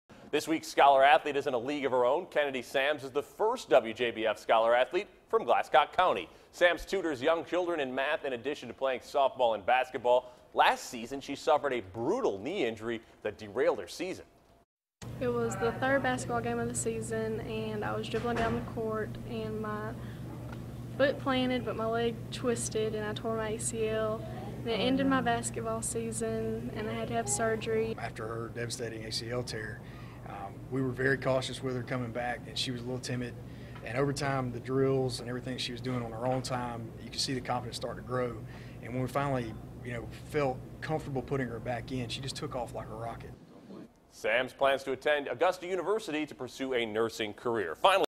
Scholar Athlete: Glascock County's Kennedy Sams
This week's Scholar Athlete is in a league of her own, as Kennedy Sams is the first-ever WJBF Scholar Athlete recipient from Glascock County High School.